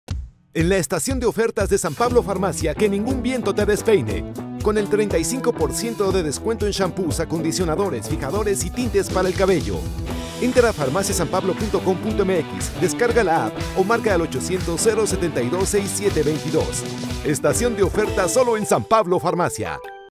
Estación de ofertas | Productos para el cuidado del cabello 🧔🏻👩🏻 | San Pablo Farmacia
Productos para el cuidado del cabello

Mantén tu cabello fuerte y sano, aprovecha la Estación de ofertas de San Pablo Farmacia para disfrutar de descuentos únicos en shampoos, acondicionadores, fijadores y tintes. Explora otras categorías donde también tendremos ofertas exclusivas para ti.

¡Cuida tu salud y la de tus seres queridos!